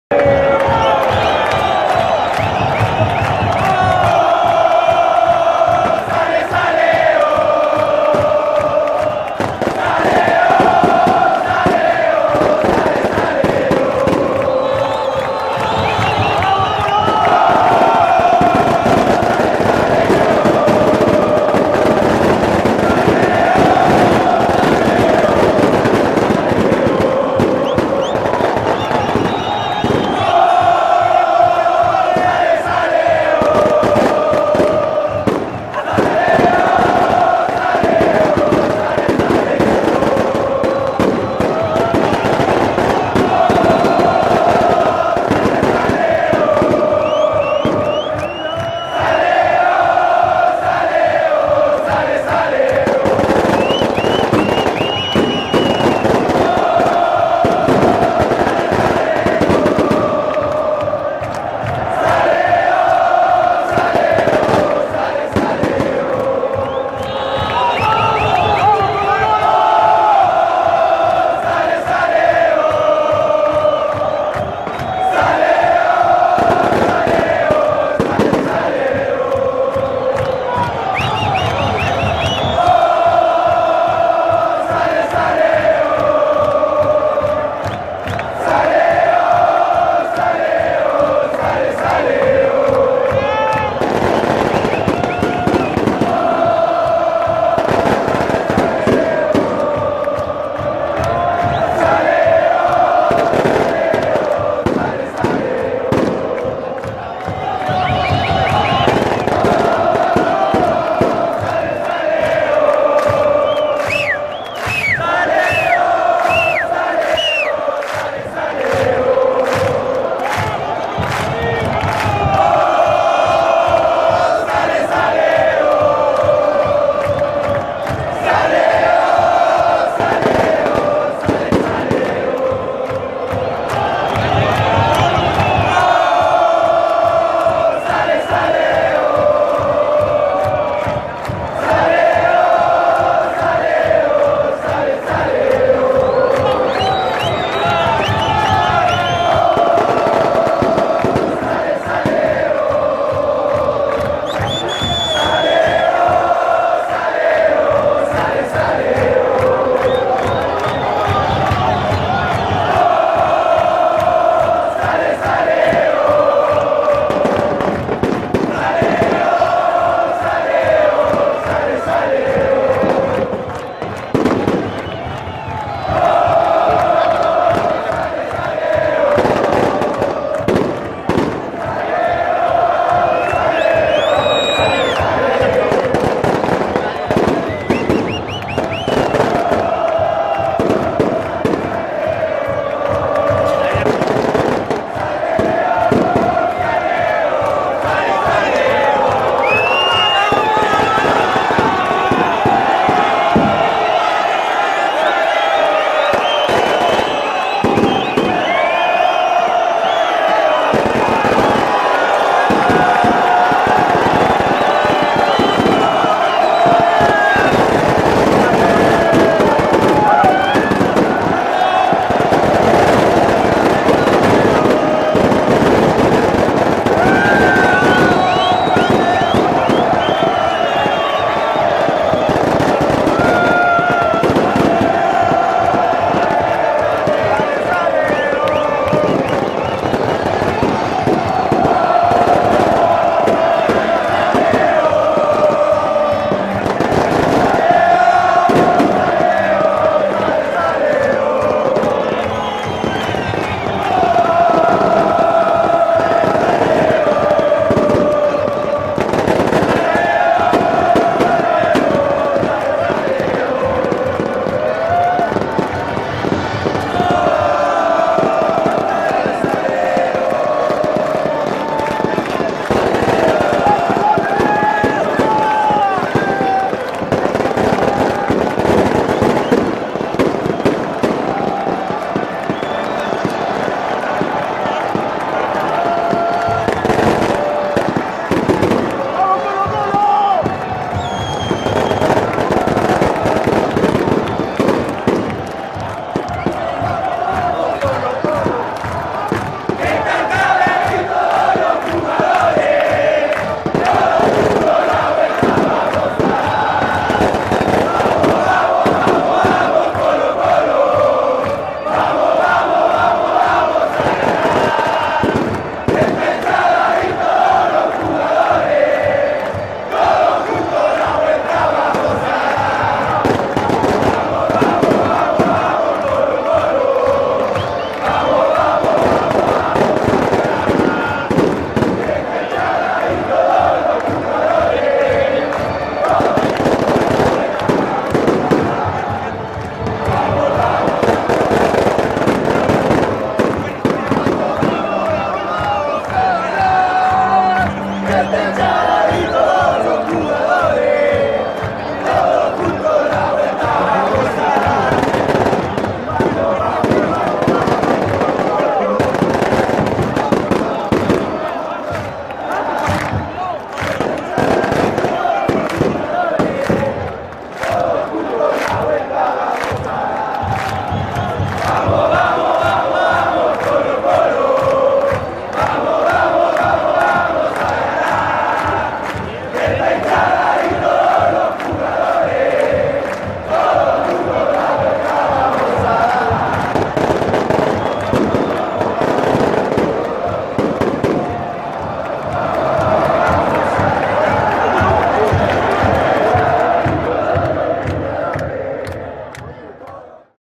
[360º] Salida Superclásico Colo Colo - U. de Chile - 02/10/2016
¿No fuiste al estadio? Tranqui. Pincha play, ajusta la calidad al máximo y siéntete en el Monumental. Acá te dejamos la salida de los equipos al terreno de juego.